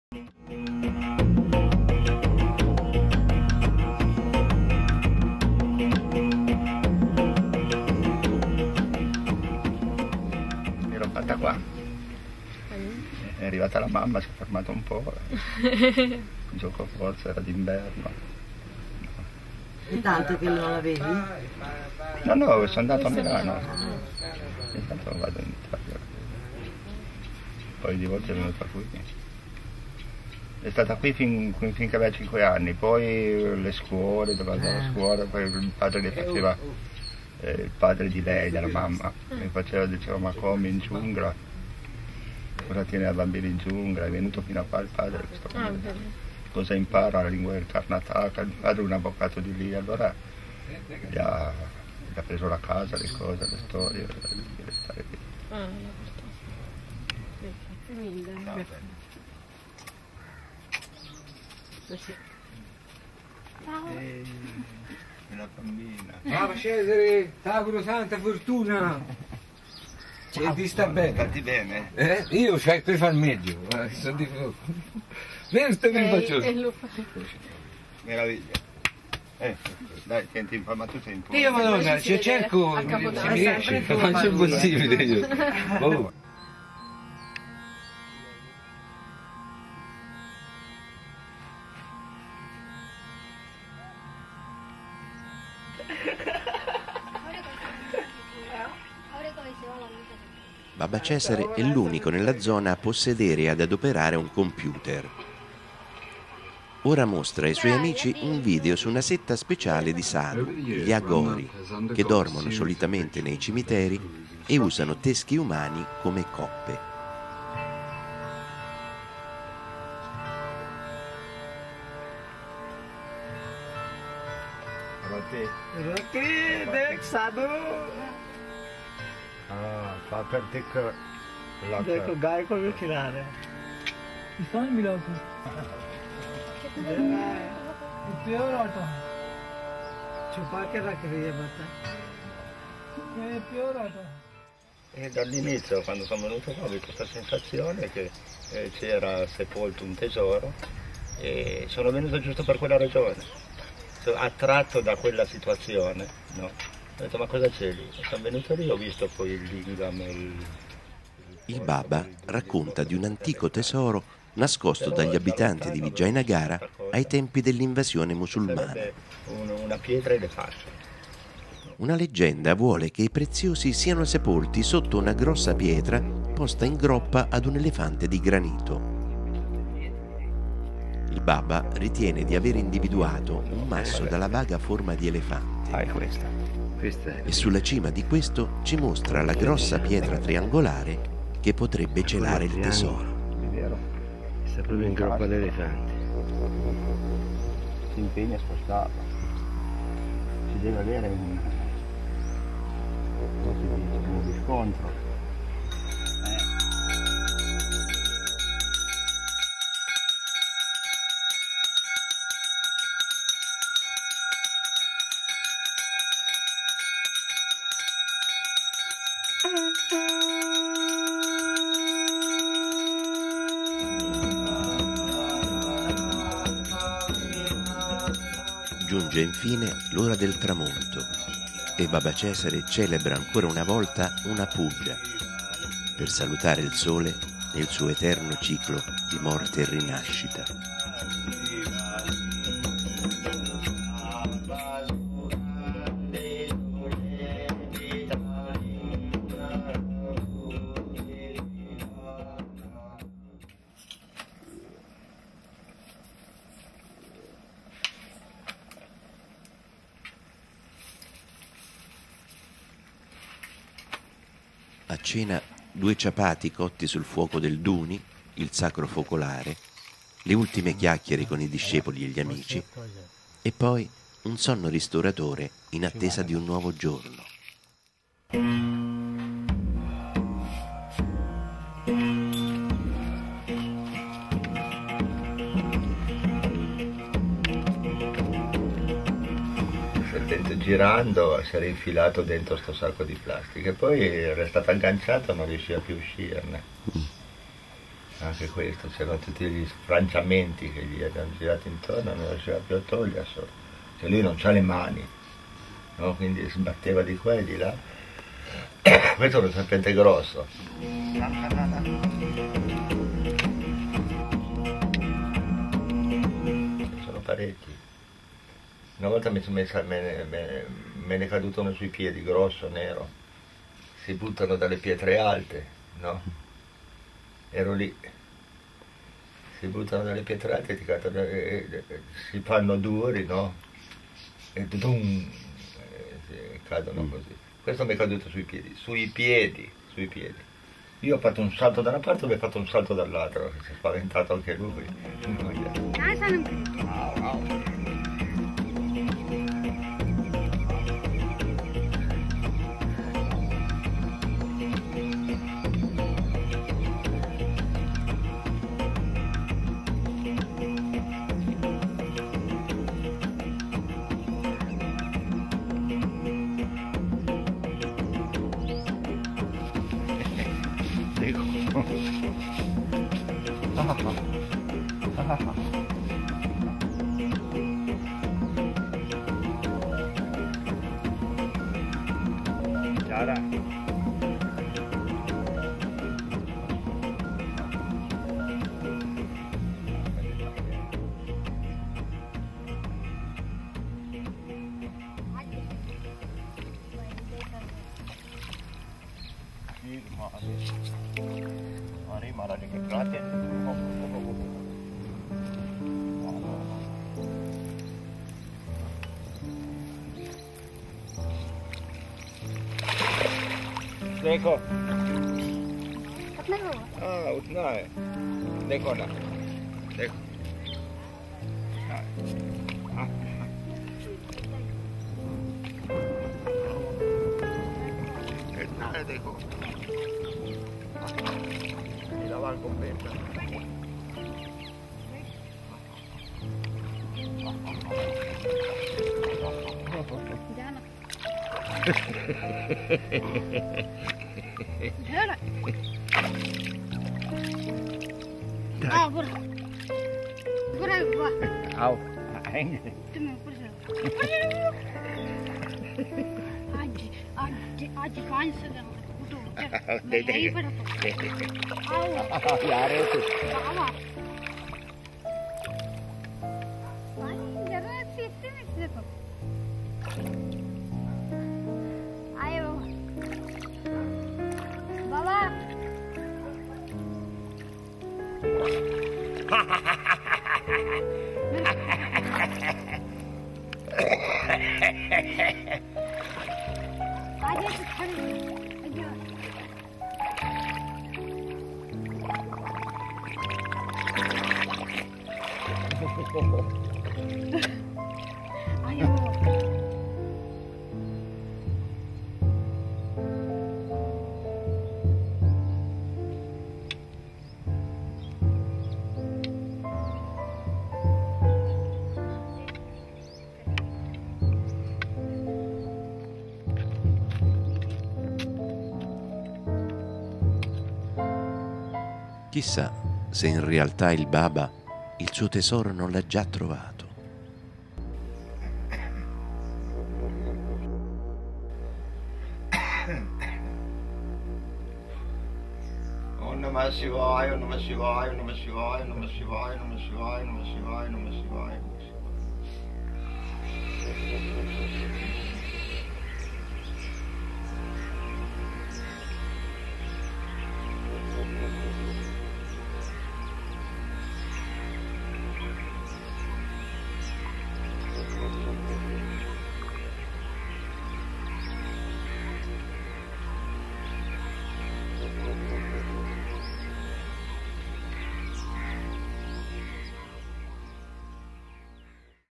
Il tesoro del Baba 3° parte (2012) Copy
La terza ed ultima parte del video girato ad Hampi, all'ashram di Baba Cesare, da Alessandro Antonaroli